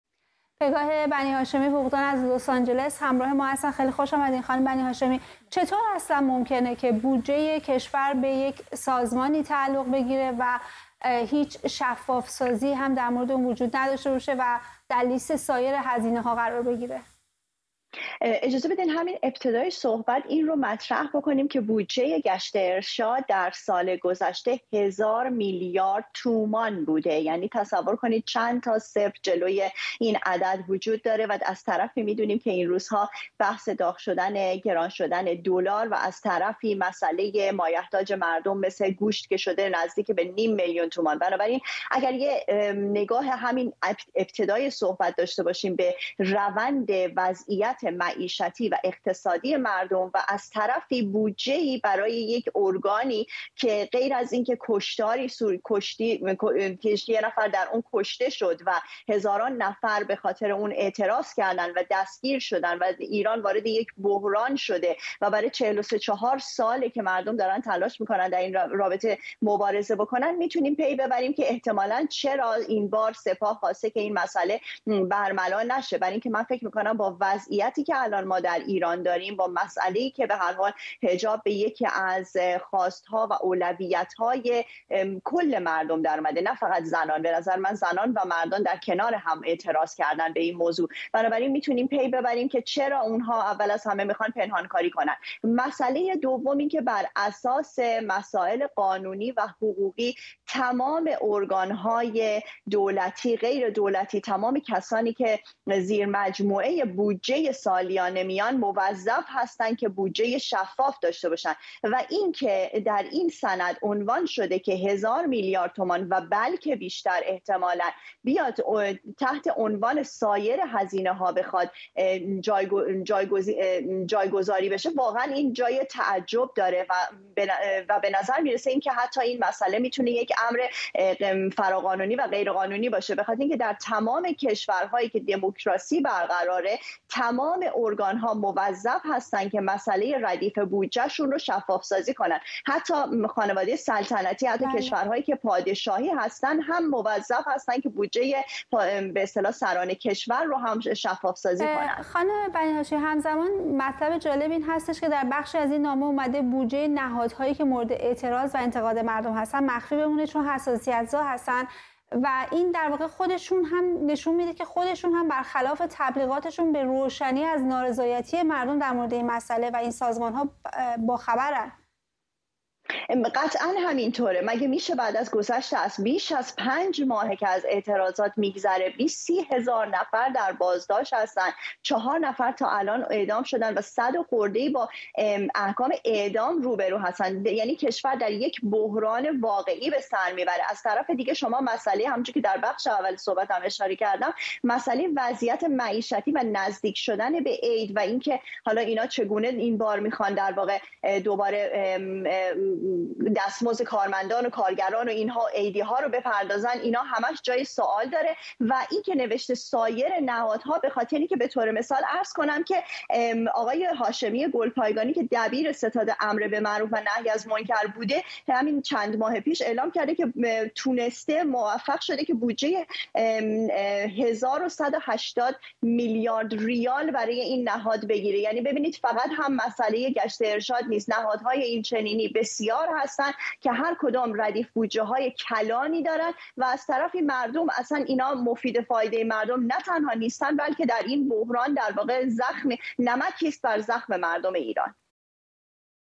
یک هزار و ۱۸۰ میلیارد ریال، بودجه امسال گشت‌ ارشاد است و سپاه درخواست کرده تا این بودجه پنهان بماند
یک هزار و ۱۸۰ میلیارد ریال، بودجه امسال گشت‌ ارشاد است. بر اساس یک سند محرمانه، سپاه درخواست کرده بودجه گشت ارشاد در قالب «سایر هزینه‌ها» نوشته شود. سپاه می‌گوید این خواسته برای «جلوگیری از تنش‌های احتمالی» مطرح شده است

گفت‌وگو با پگاه بنی‌هاشمی، حقوقدان